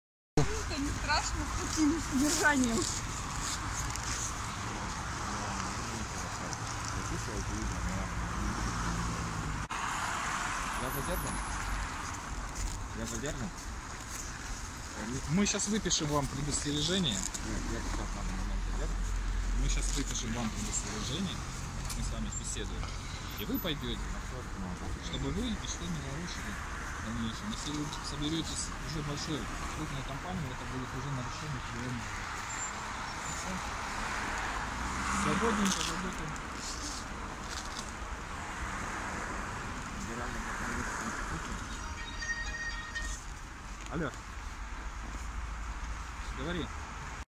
Прогулка Ачинца
Прогулка ачинца была прервана полицией